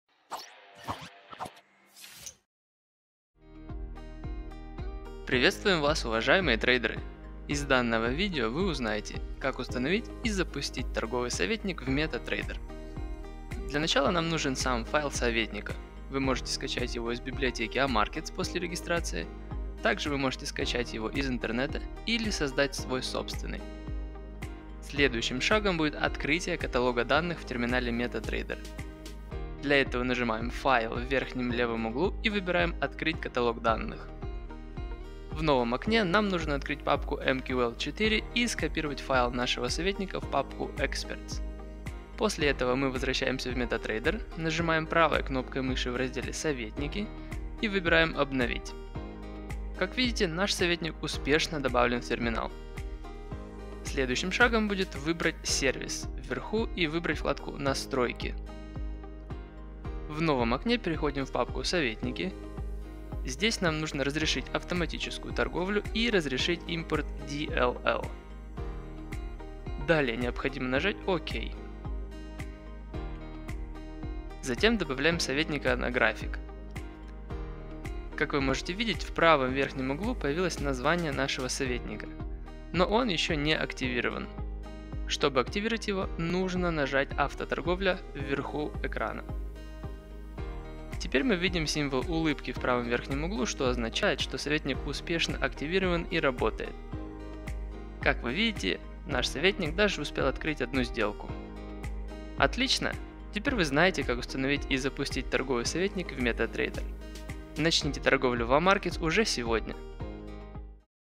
Как установить и активировать торговый советник в MetaTrader | AMarkets
Из этого видео вы узнаете, как установить и активировать торговый советник в MetaTrader.